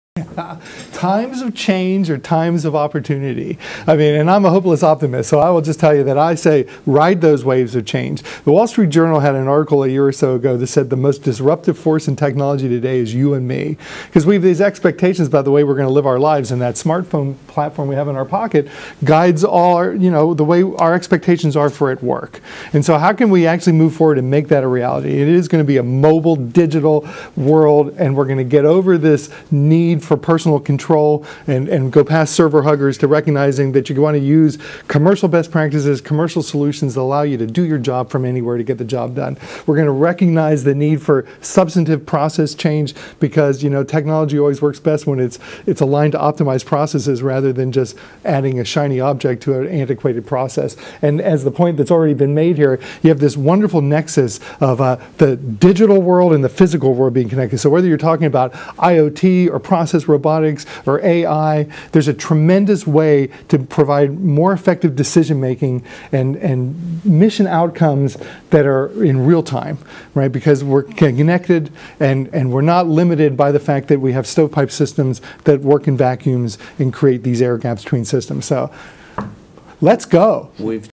FEF IT Modernization Vision Deloitte May 2018
Deliotte's Dave Wennergren on Future Vision in IT Modernization in Government-Federal Executive Forum Program (May 2018)